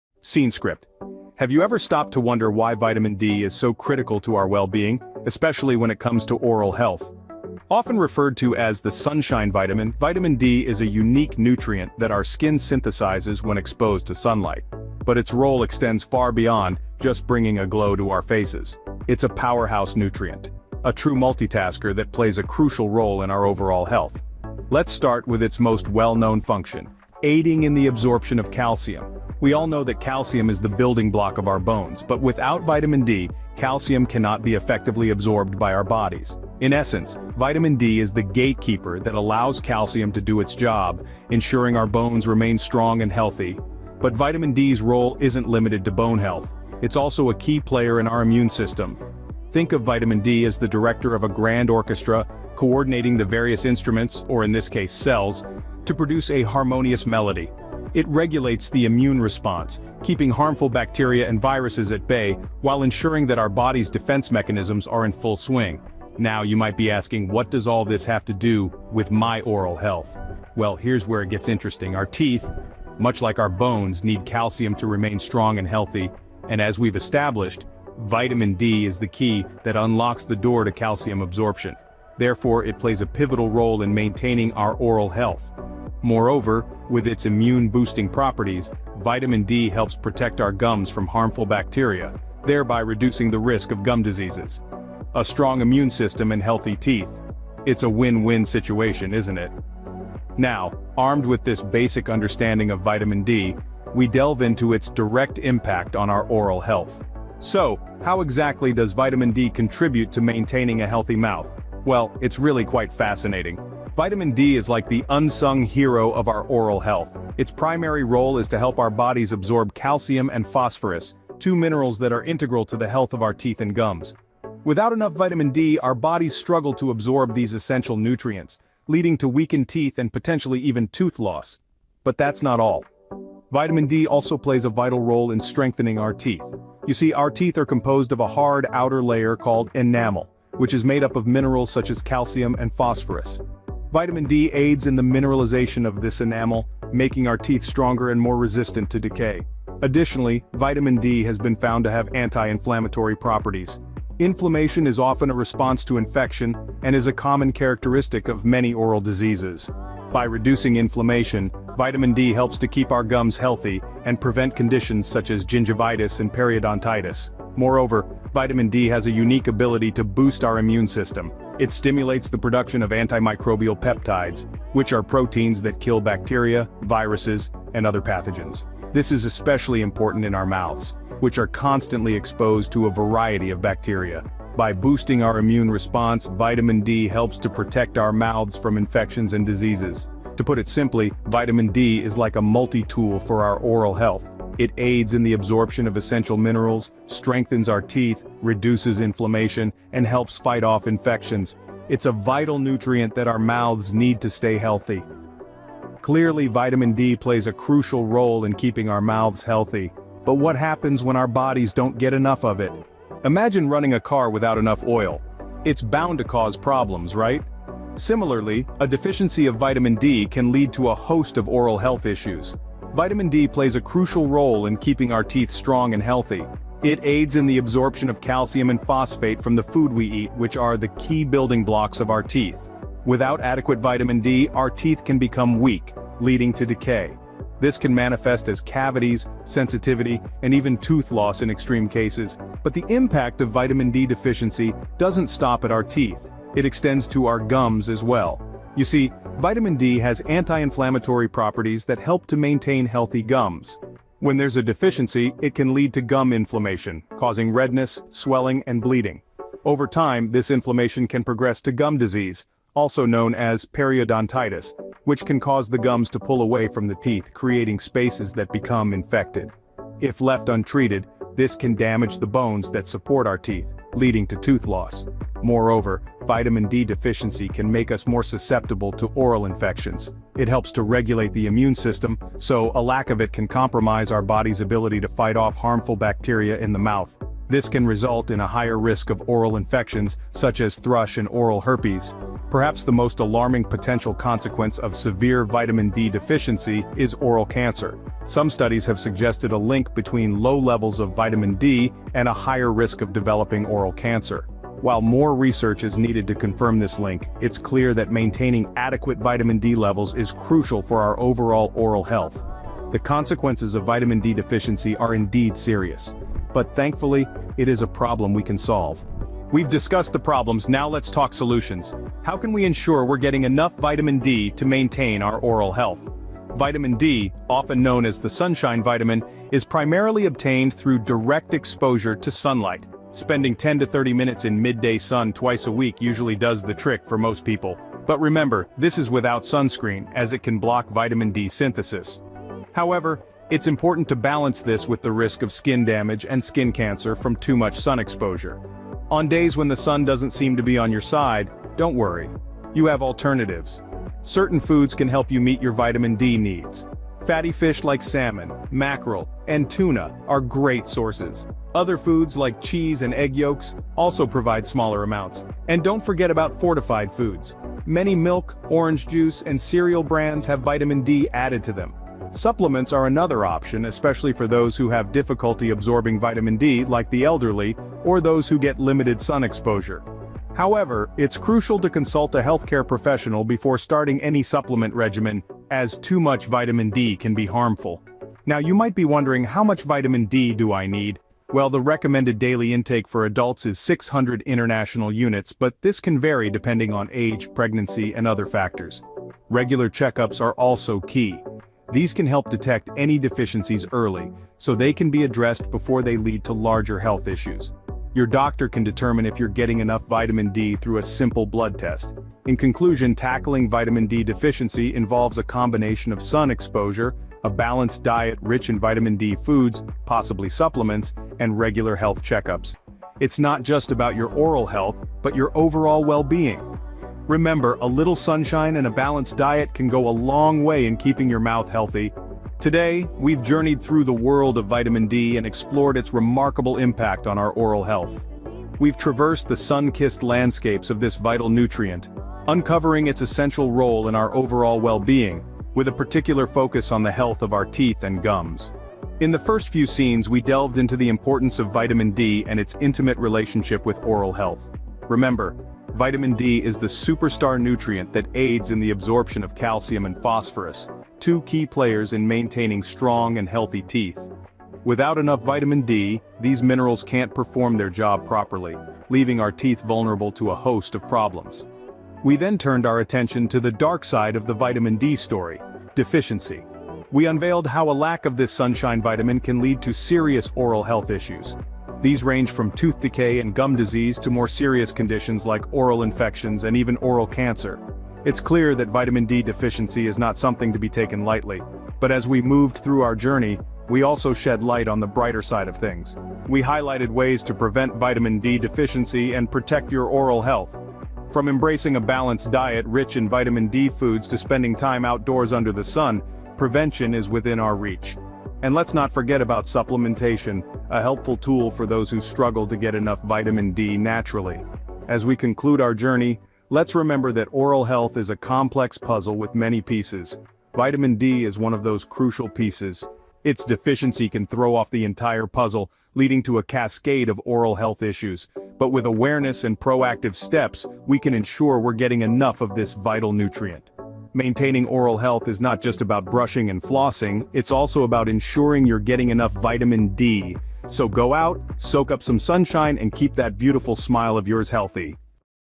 The Hidden Impact: Vitamin D Deficiency and Oral Health
Join us in this eye-opening video where we delve deep into the fascinating link between Vitamin D deficiency and oral health. Backed by cutting-edge scientific research, we reveal compelling evidence that will surely leave you amazed. Discover why proper Vitamin D levels are crucial for maintaining a healthy smile and how this vital nutrient plays an essential role in your overall dental well-being.

OUTLINE:

00:00:00 The Importance of Vitamin D
00:01:47 Vitamin D and Oral Health
00:03:32 Vitamin D Deficiency and Oral Health Problems
00:05:19 Preventing Vitamin D Deficiency
00:07:12 Recap and Conclusion